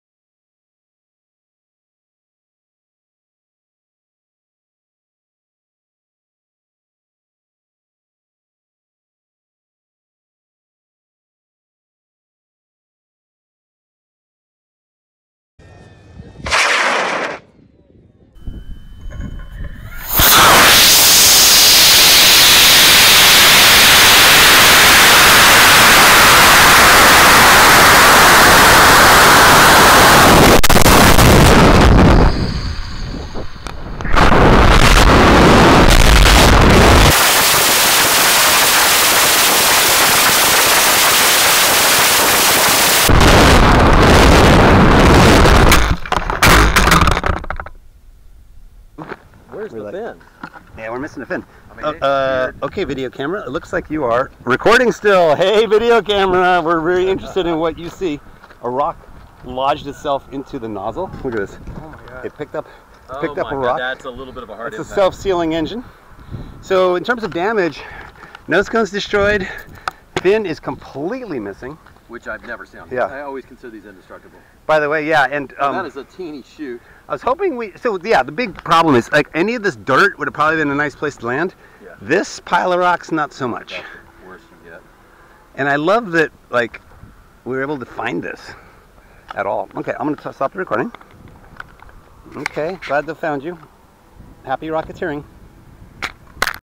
My Minimum Diameter 54mm Green Rocket Launch
On the super high thrust impulse Cesaroni J1520 Vmax motor with a 0.7-second burn. With drone footage and on-rocket video cam, at DairyAire 2023 weekend launch event. Photos